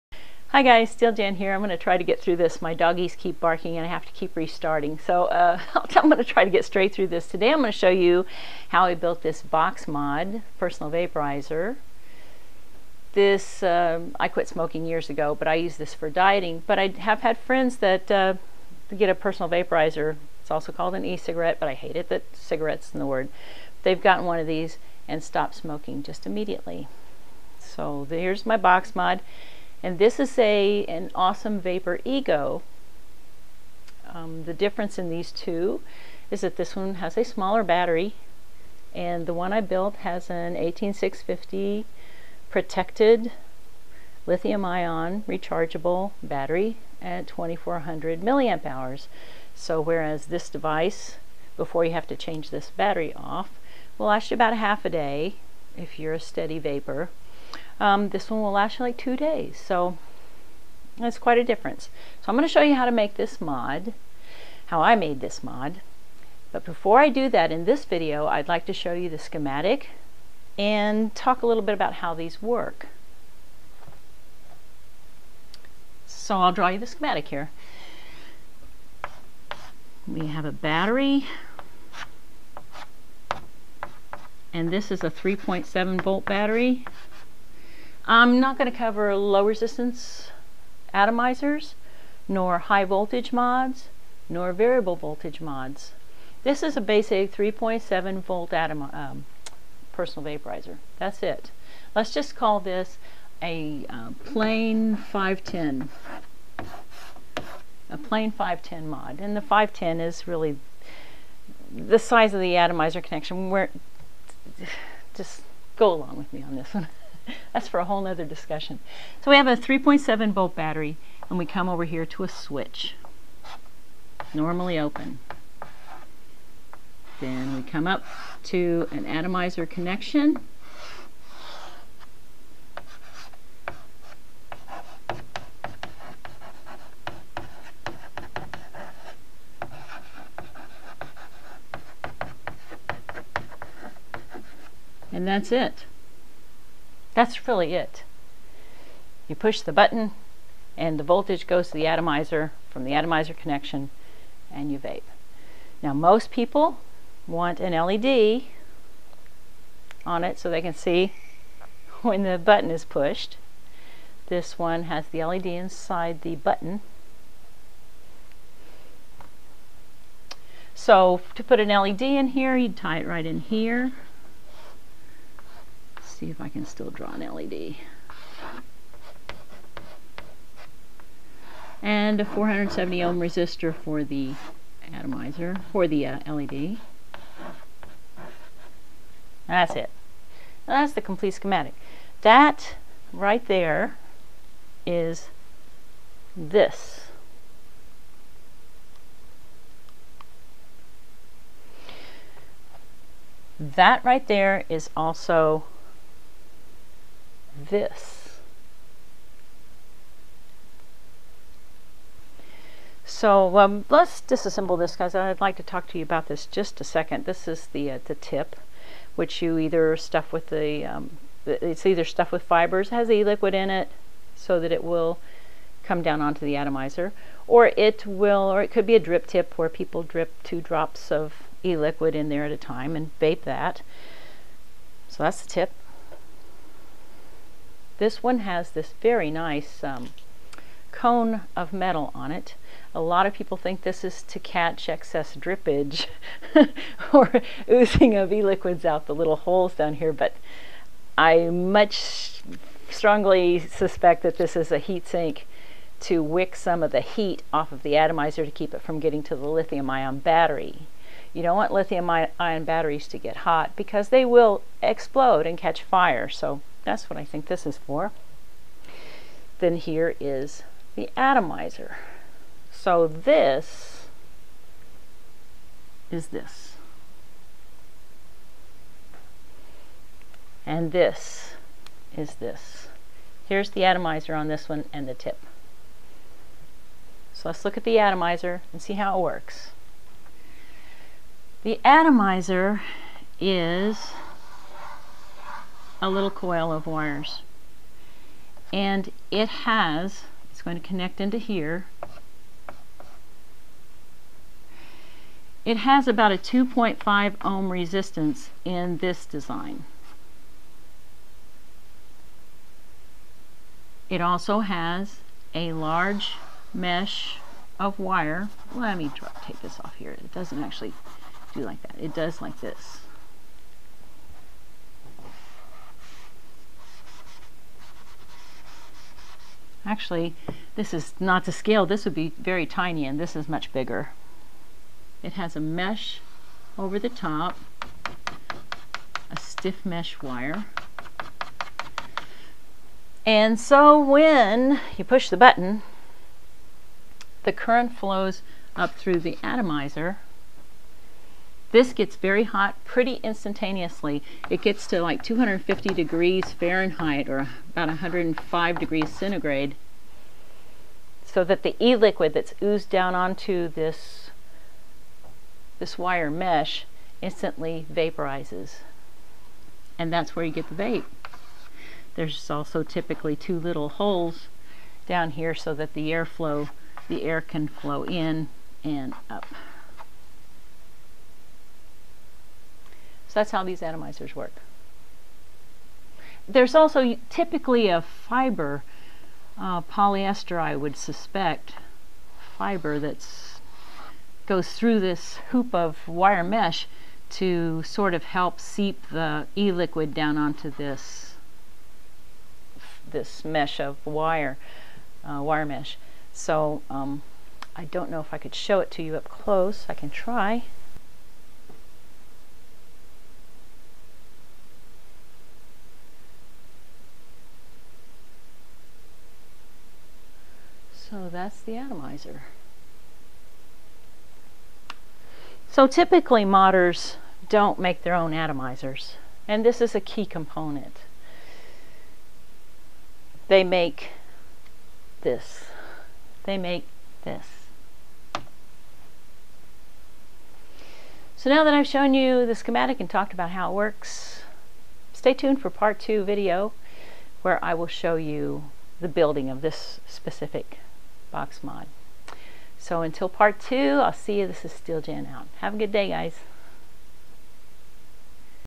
Making an 18650 Personal Vaporizer Box Mod Part 1 Schematic and How it Works
Before showing the making of my 18650 Box Mod, I cover the schematic and how the personal vaporizer works.  I also show the AwesomeVapor Ego, a great PV that I bought from awesomevapor.com -- great folks, excellent service, I highly recommend them, and their Ego.

All the parts used in my mod came from:
Radio Shack - the box, pushbutton, and battery holder (that I cut up for piece parts) - I could have gotten these from madvapes but happened to stop by my local Radio Shack for the fun of it.
madvapes.com - the LED, resistor, battery-atomizer connector
batteryjunction.com - They have a $30 deal on 2 protected 2400maH 18650 batteries and charger.  Wow!
Target - Hot glue
Harbor Freight - JB stick Epoxy putty

Be extremely careful working with lithium ion batteries.  ALWAYS use a PROTECTED Lithium Ion battery so that it will not overcharge or under-volt because they will explode and burn.  Recharge these batteries outside on concrete under constant supervision in case they explode and burn while recharging.  Unplug the charger and remove the battery(ies) as soon as charging is complete.

Please do NOT attempt to make a pv e-cig mod unless you are an experienced electronic technician or a well-seasoned electrical-mechanical engineer.  Each solder connection is critical, make it mechanical and then solder.  Always remember, exploding lithium ion batteries can burn down your house and/or kill you and the ones you love!!!

This video is for educational curiousity interest only.  Mod at your own risk.

Filmed with a Kodak Zi8
Edited in Windows 7 with Windows Live Movie Maker